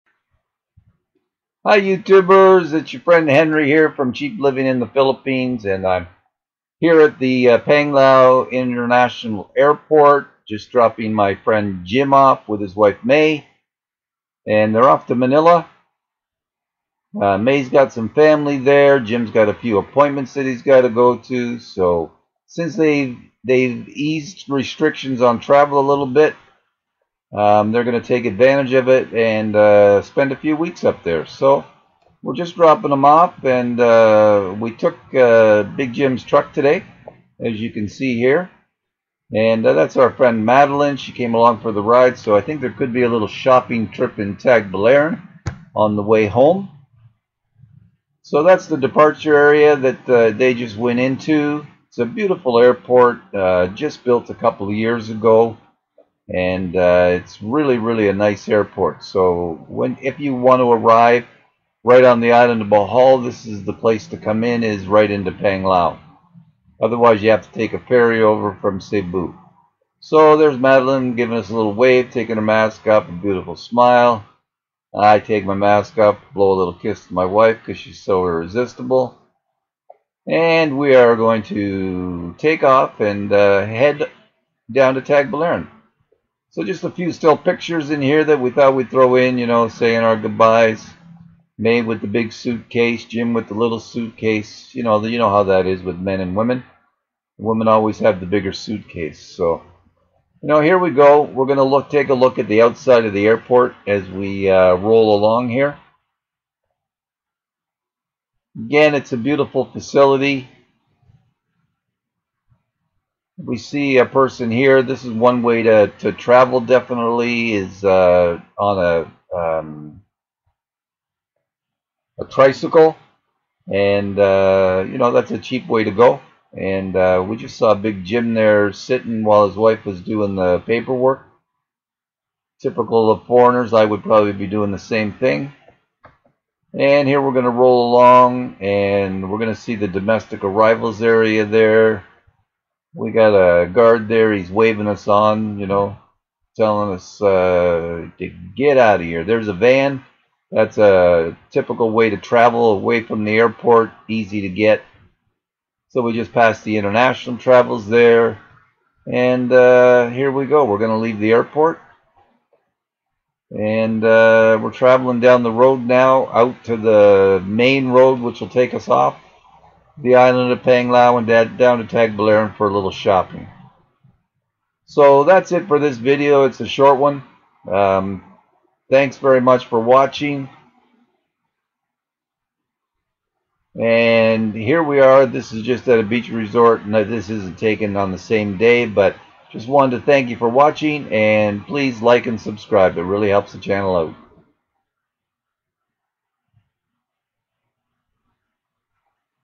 Panglao Island International Airport, Bohol
Dropped some friends off at the airport today so we took a short video of the outside of the airport. Beautiful facility.